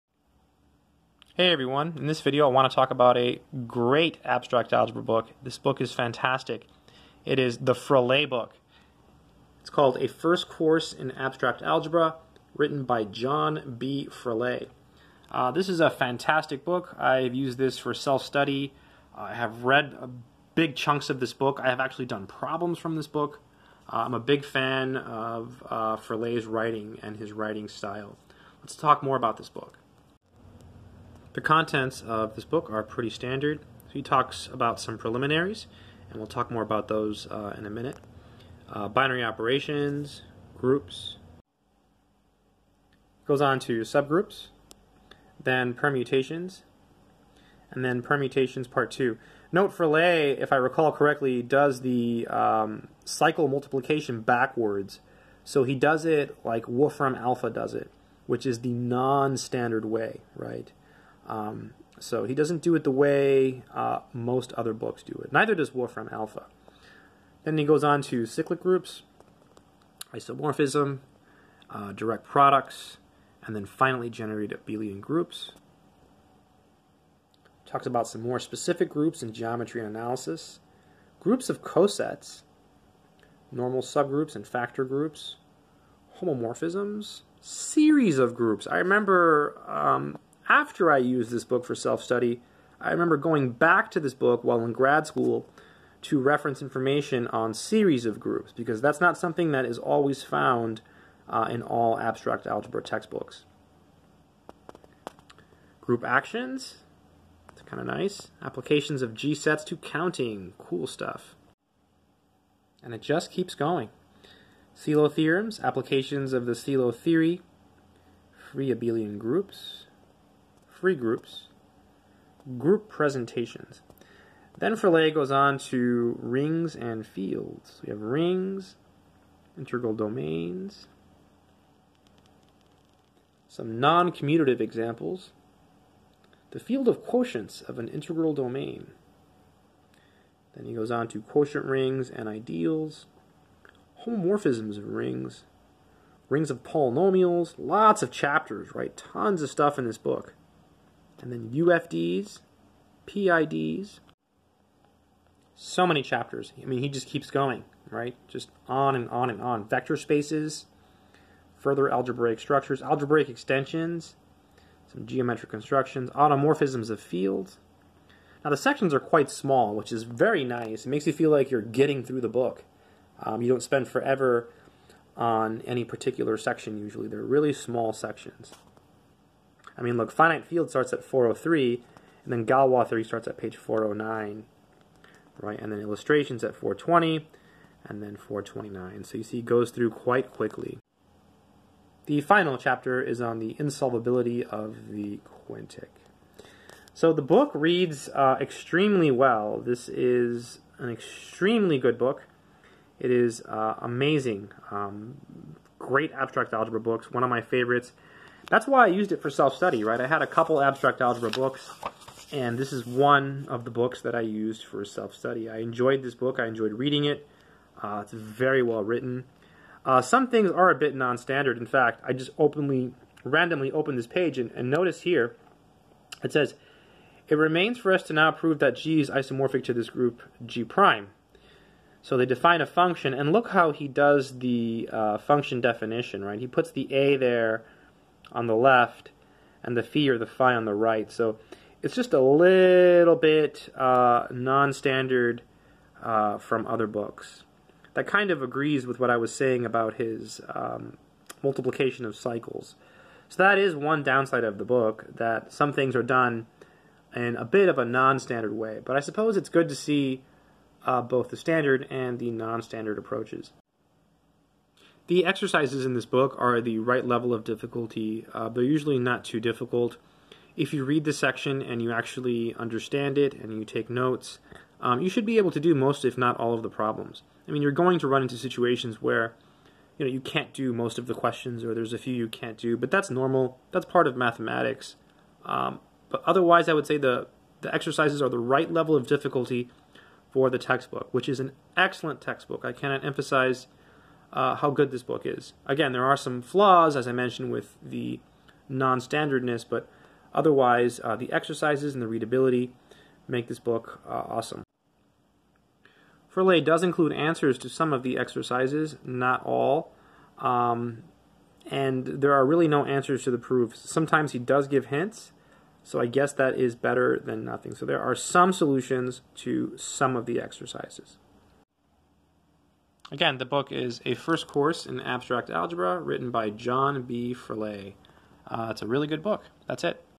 Abstract Algebra Book for Self Study
This is an Intermediate Level Abstract Algebra which I used extensively for self study. I bought this book and read a lot of it on my own to help learn more abstract algebra.

The book is called A First Course in Abstract Algebra and it is written by John B. Fraleigh.

This is the book on amazon

This book is not the ultimate beginner book but it is not a super advanced book either. I would say this is a "middle of the road" type abstract algebra book. It's written at a slightly higher level than the most beginner books out there and also contains more information, so it is totally worth having if you are serious about abstract algebra. In this video I carefully go over the book and talk about a few things worth mentioning about this book.

Please leave any comments or questions in the comment section below. Thank you:)